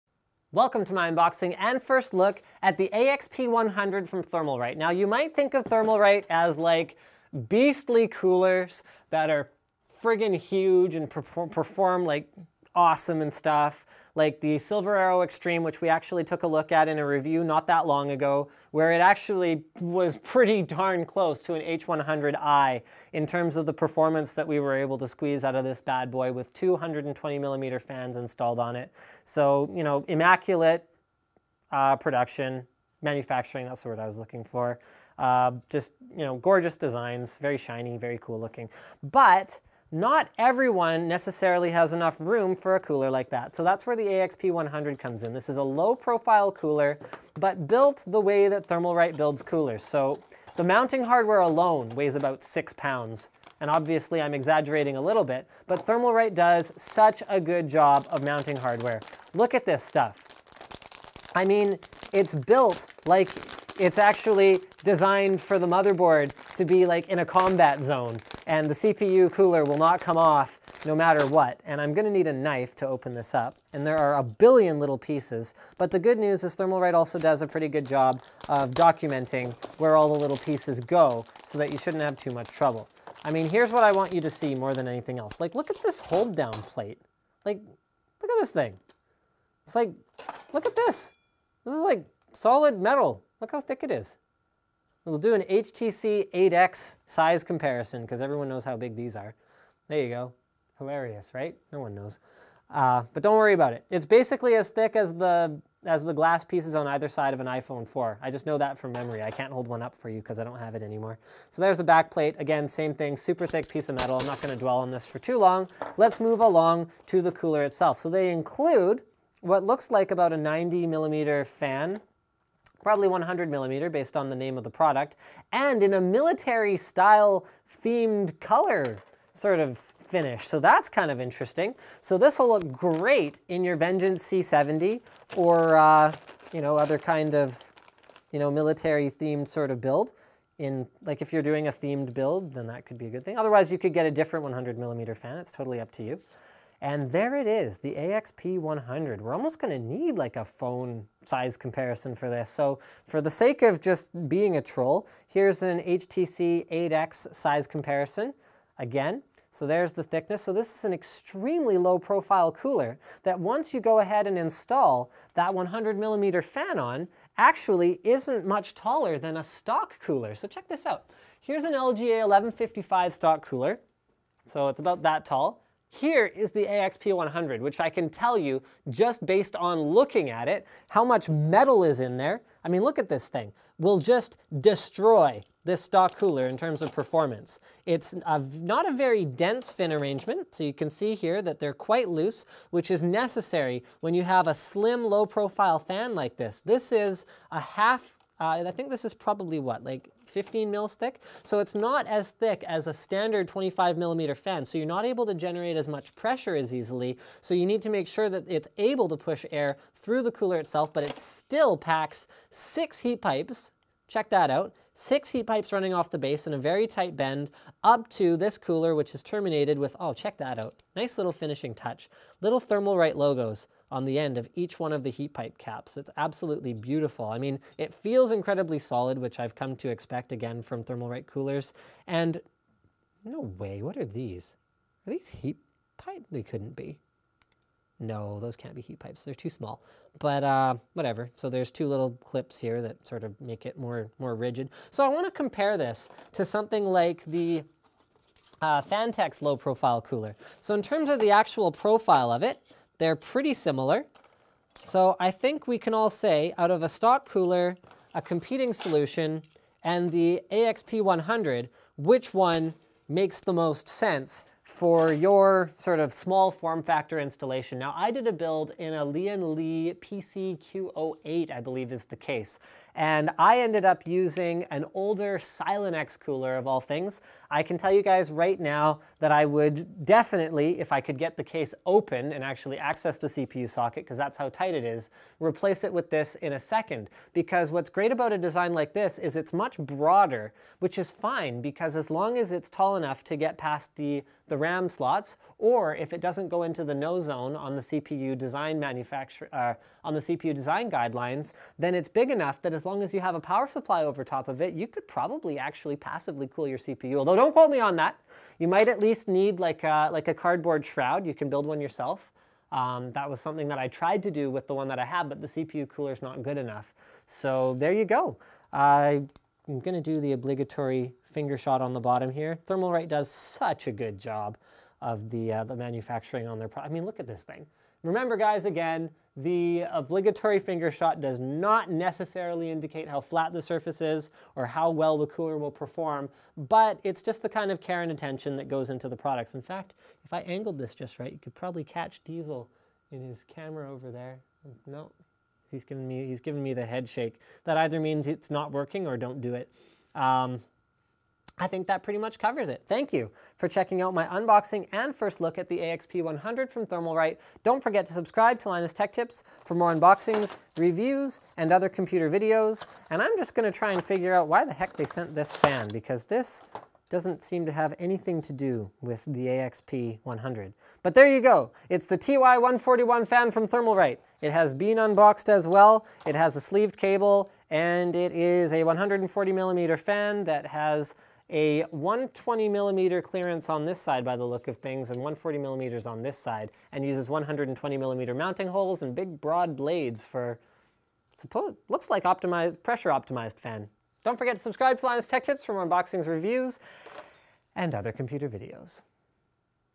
Thermalright AXP-100 Low Profile Heatsink Unboxing & First Look Linus Tech Tips
The AXP-100 is perfect for small form factor builds where you still want silence and performance. Thermalright quality and mounting hardware is among the best in the industry. We used this for our ITX Titan build which you can see below: